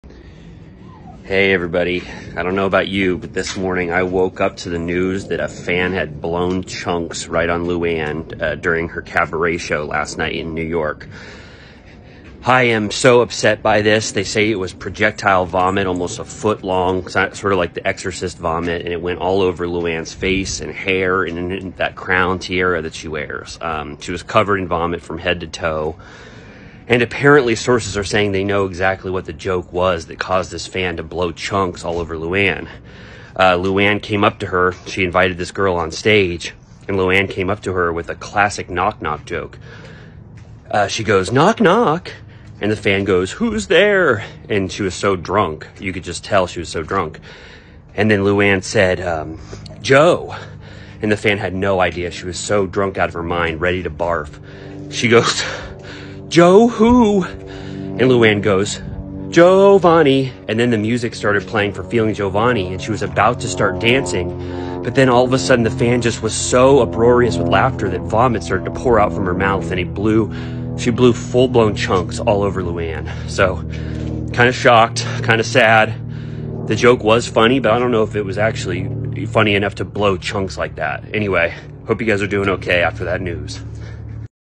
Fan Throws Up at RHONY star Luann de Lesseps Cabaret
Exclusive: here’s that joke that caused a fan to “blow chunks” at Luann de Lesseps’ cabaret show.